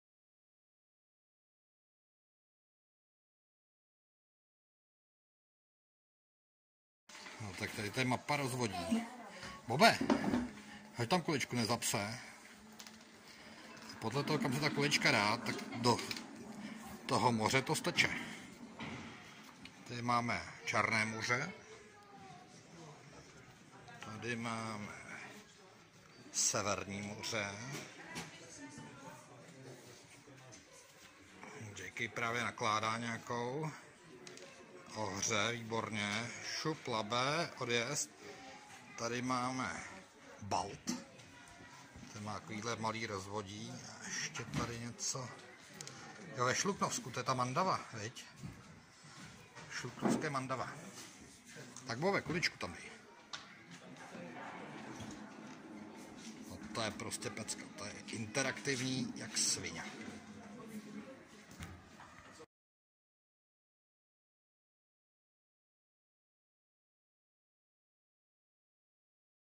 Expedice Azbest: Vodní dům Hulice, 25.7.2020
Závěr moravské epopeje u strýce Azbesta proběhl ve Vodním domě Hulice u přehrady Želivka. JayKaye to vcelku bavilo, hlavně venkovní ráchací část.